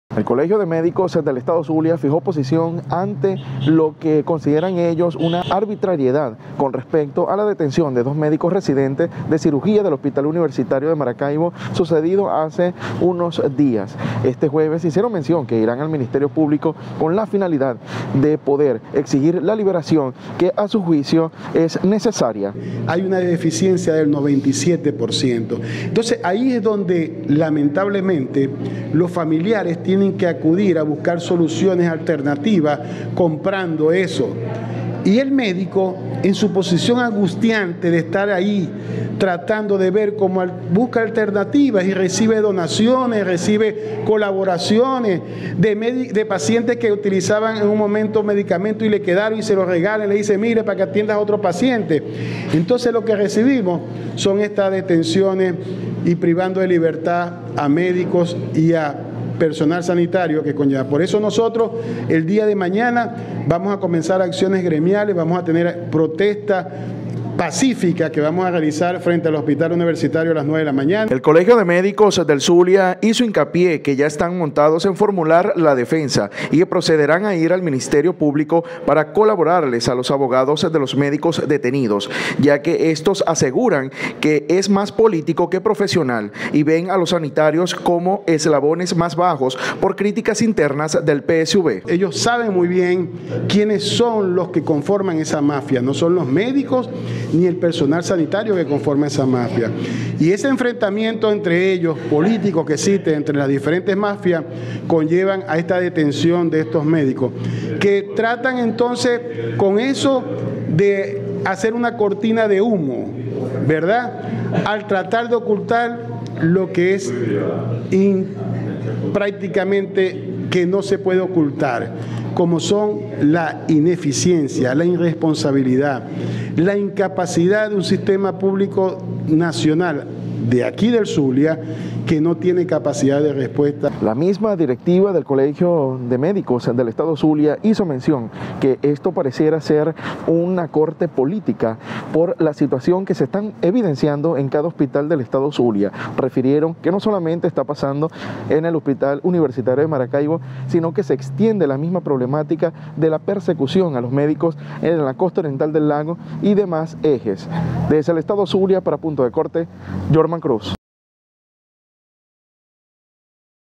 ZULIA | Colegio de Médicos fijó posición y rechaza detención de residentes en Maracaibo
Colegio de Médicos fijó posición y rechaza detención de residentes en Maracaibo